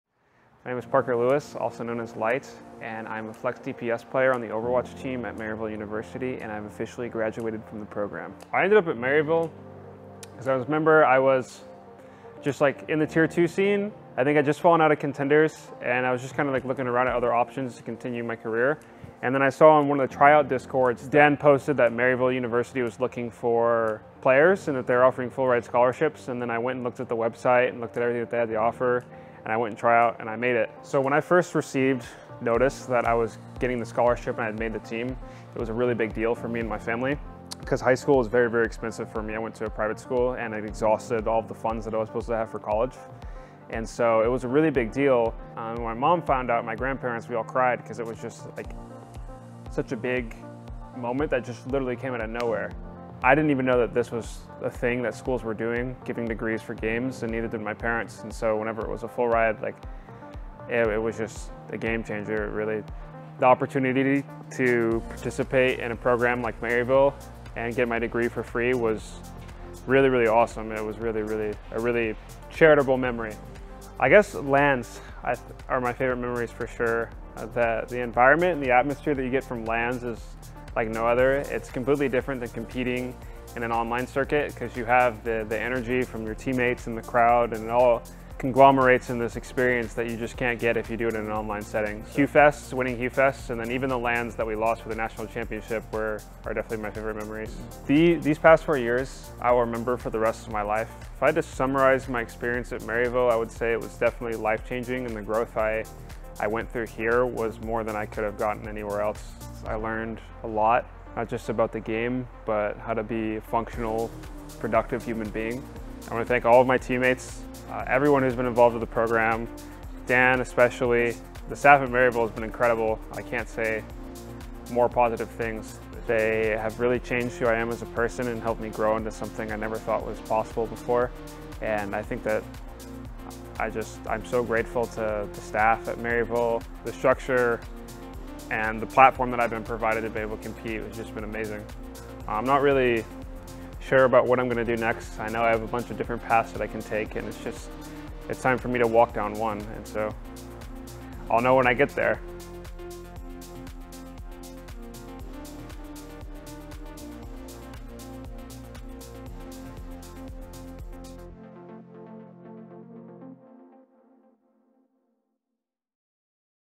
Thank You, Parker
Today we say goodbye to one of the founding members of the Maryville Overwatch program, Parker "Lyte" Lewis. He has been an instrumental piece of the team and we can't thank him enough for everything he's contributed over the last 4 years.